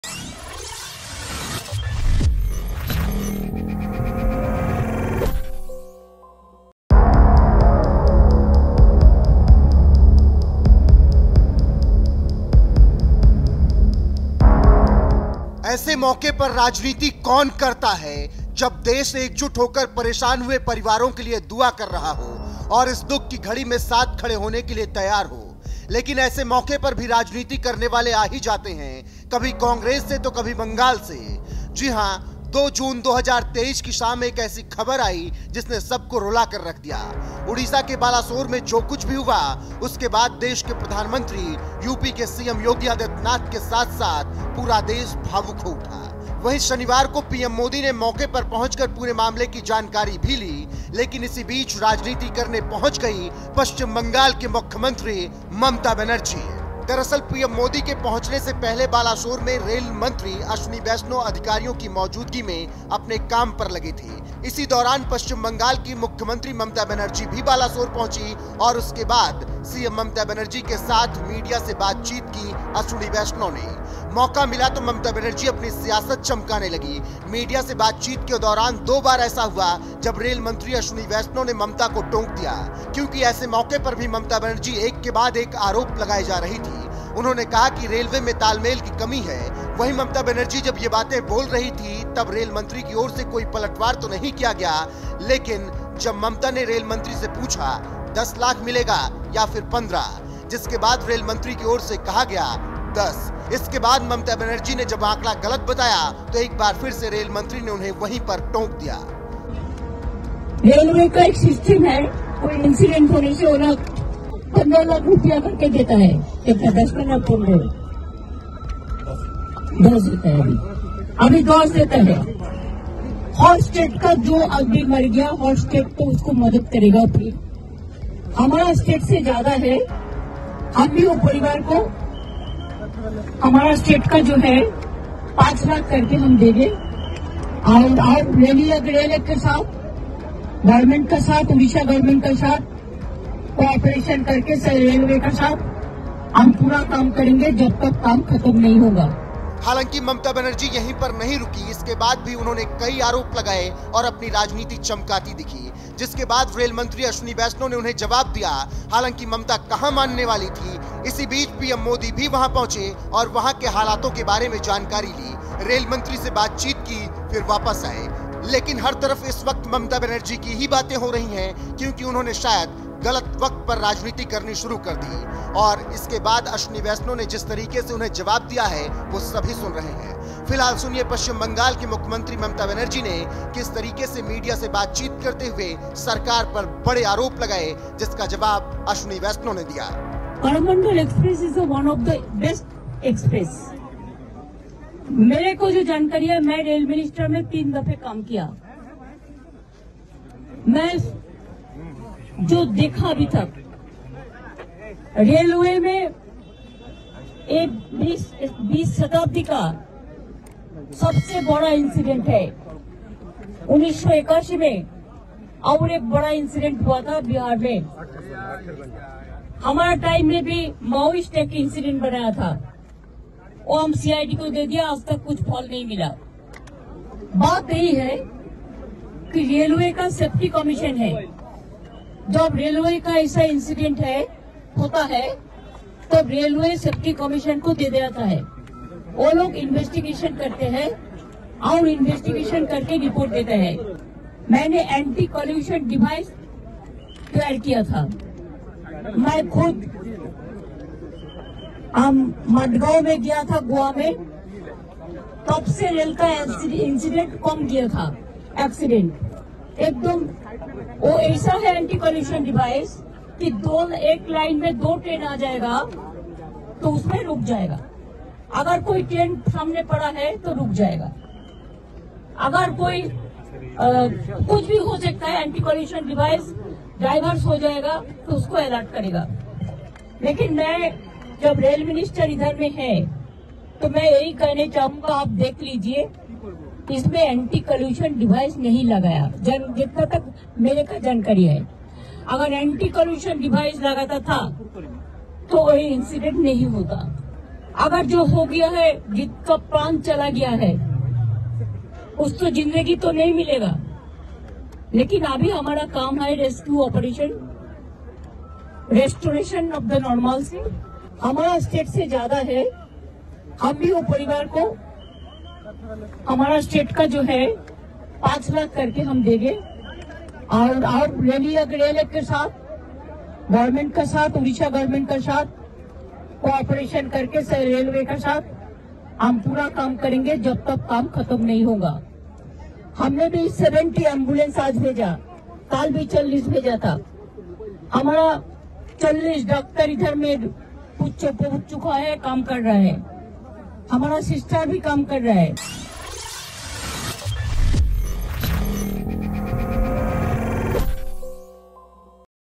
बकवास कर रही थी ममता बनर्जी, मोदी के मंत्री ने 2 मिनट में हेकड़ी निकाल दी
पश्चिम बंगाल की मुख्यमंत्री ममता बनर्जी ने रेल मंत्री अश्विनी वैष्णव के सामने राजनीति शुरु कर दी, जिसके बाद उन्हें जवाब भी दिया गया, जानिए क्या है पूरी खबर

rohit
uma